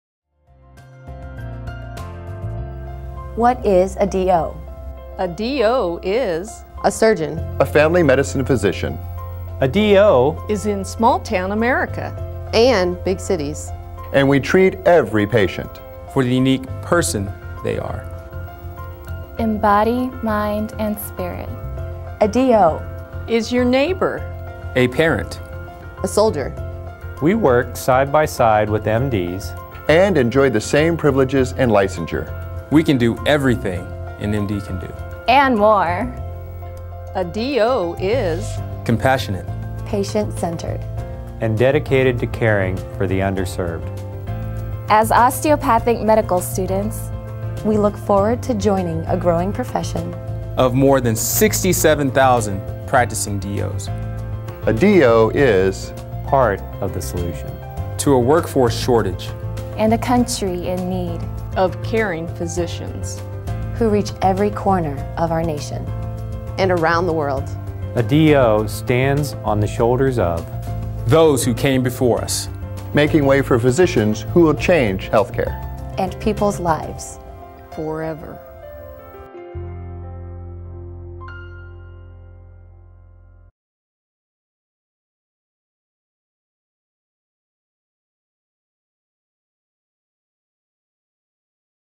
What is a D.O.?
A.T. Still University doctors and students tell you what a D.O. is.